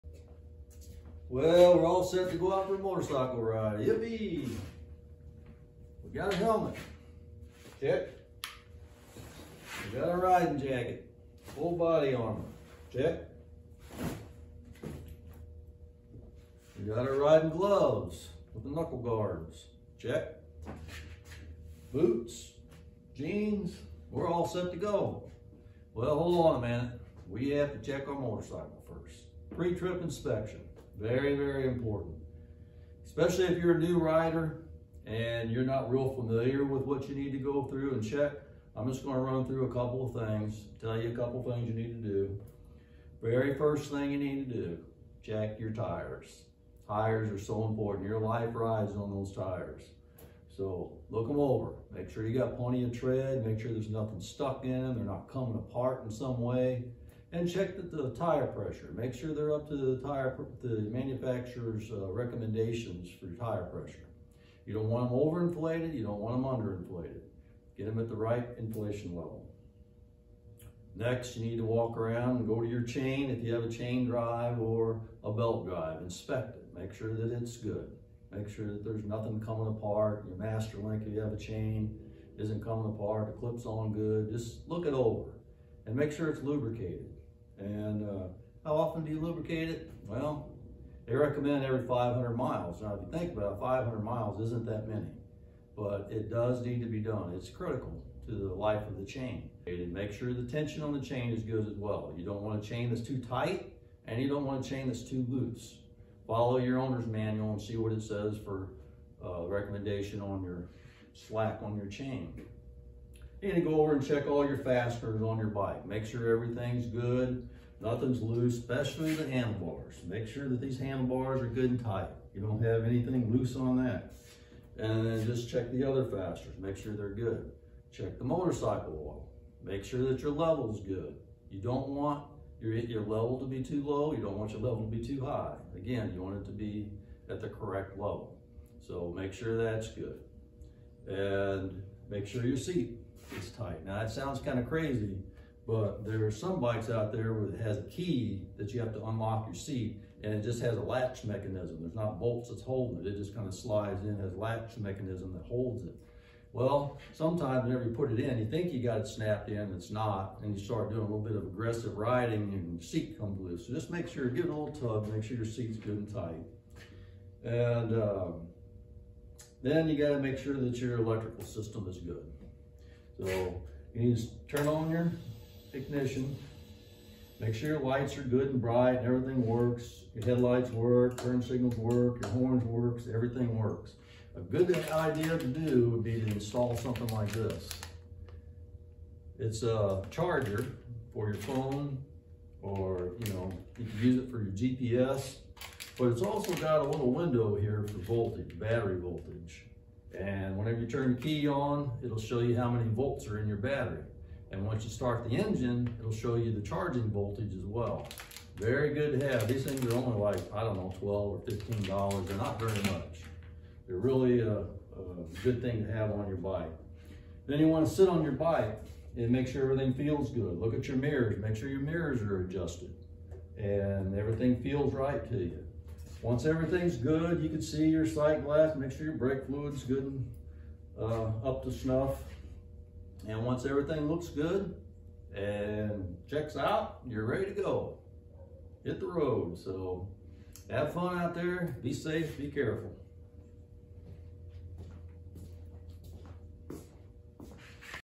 Motorcycle pre trip inspection
Motorcycle pre trip inspection is a very important part of riding safety as well as wearing the proper riding gear. Whether you are a beginner or a seasoned rider, it's good to perform your own inspection of your bike. One other thing that I should have included in the pre-trip inspection is to do a rolling check of your front and rear brakes as well. Make sure you have a good feel and pressure in your brakes and to make sure they stop properly. Be safe out there and ride safe and watch out for distracted drivers! Have fun!

The reuse of this video without written permission of the owner is not allowed.